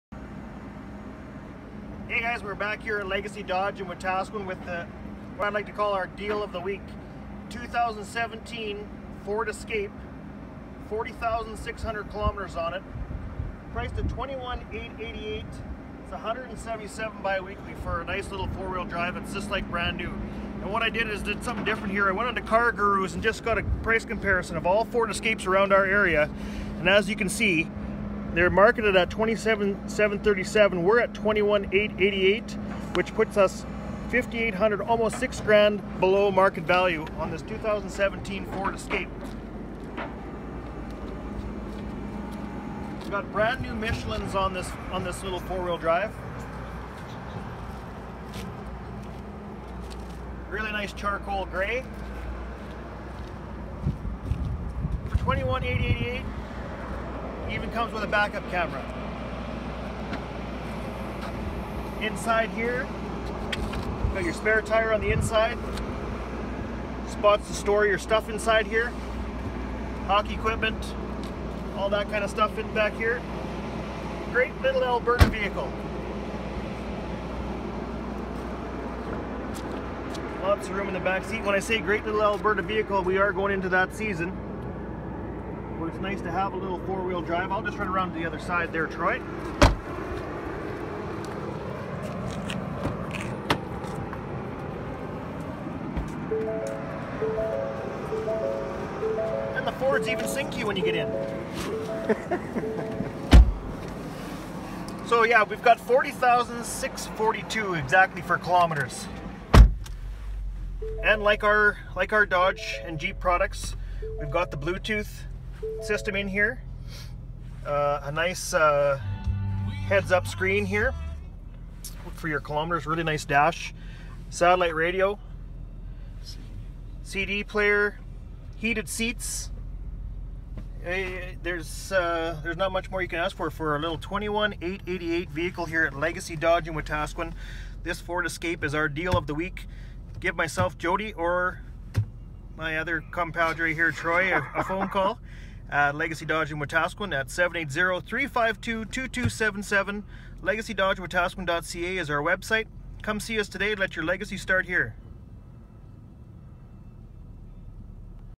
2017 Ford Escape | $21,888 - Payment: $177/Bi-Weekly | Wetaskiwin, AB
Stock# T0361A

Come to Legacy Dodge Wetaskiwin for a test drive of this 2017 Ford Escape.

This model is on for $21,888 with payments of $177 Bi-Weekly.

Call Jody at Legacy Dodge Wetaskiwin - or stop by the dealership to see any of our other new or pre-owned models!

Legacy Dodge Wetaskiwin
3920 56 St
Wetaskiwin, AB
T9A 2B2